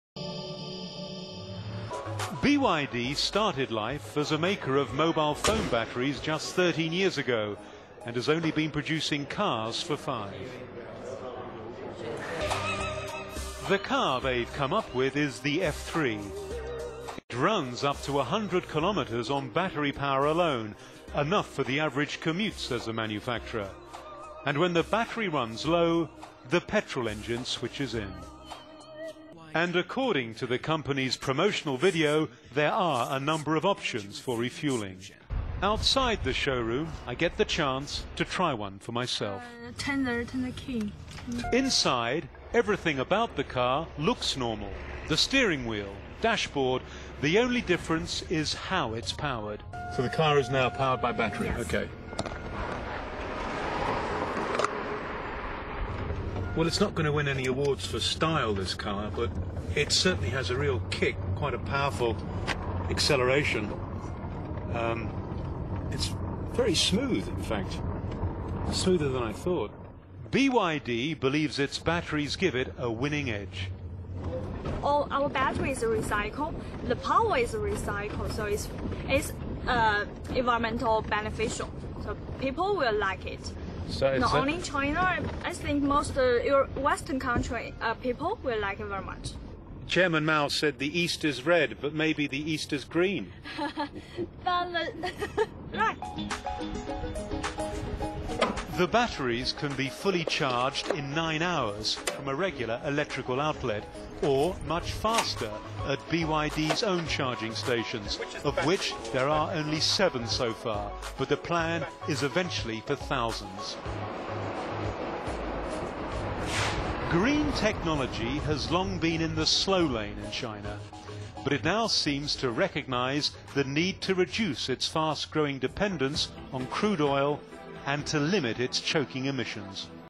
byd--汽车电动时代全球领导者
去年12月份，中国比亚迪股份有限公司(BYD Co)率先推出一款插入式双模电动汽车，领先其他实力强劲的外国竞争对手，震惊了全球汽车市场。有报道说，这家位于西安的公司正在与几家欧美汽车制造企业谈判就转让汽车电池技术磋商。如果谈判达成协议，比亚迪在电动汽车市场的领先地位将得到进一步巩固。

目前日本在混合动力汽车方面仍领先于全球，丰田和本田公司已经推出了达到实用水平的混合动力车，这种车既可以使用汽油，又可以充电。例如丰田公司的Prius和本田公司的Insight，已经在市场上取得成功。美国在这方面已经落在后面，通用汽车公司计划推出的即插充电式小型车Chevrolet Volt要到明年才会上市，而且这款车使用的可充电锂电池还是从韩国LG公司进口的。